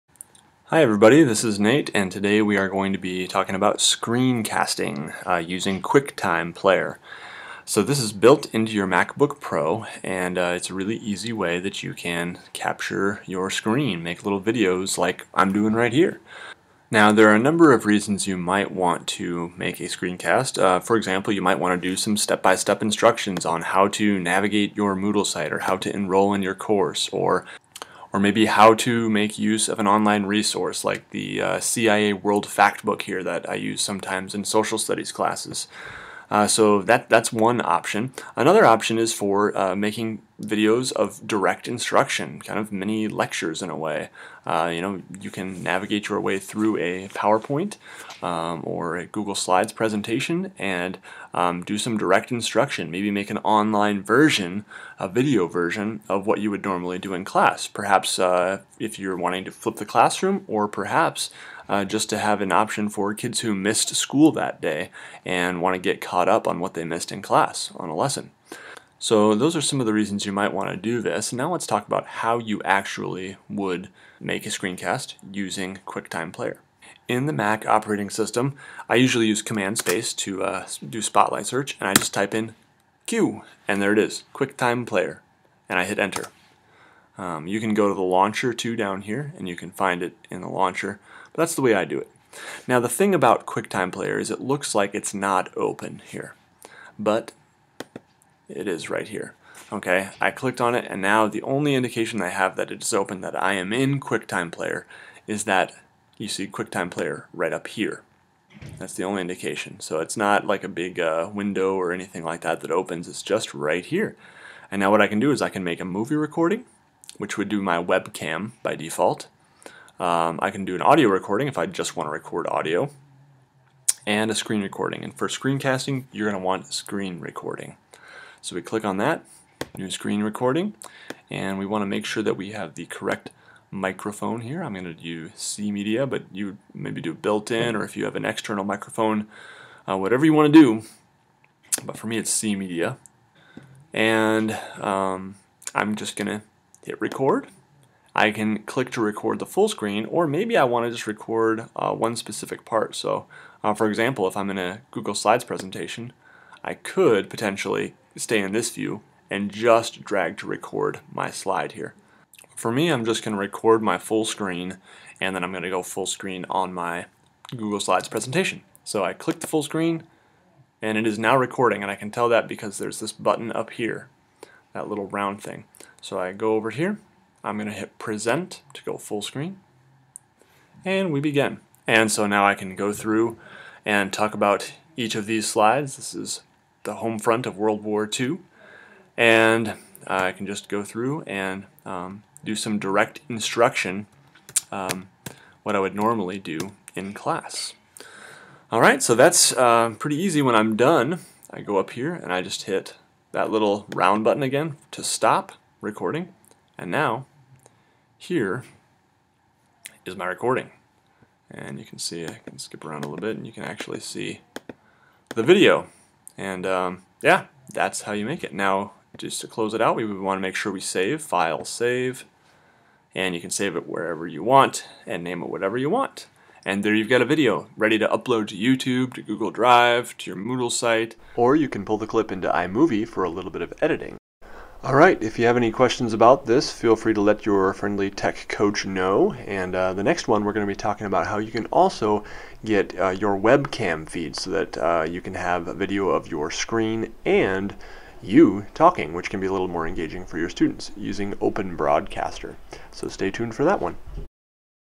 Easy Screen Recording on Mac (Quicktime Player)
This video shows you a very easy way to make a screencast using the free software built in to Mac OS: Quicktime Player.
Recording your screen can be very useful for teachers and is something you may even consider having students do occasionally.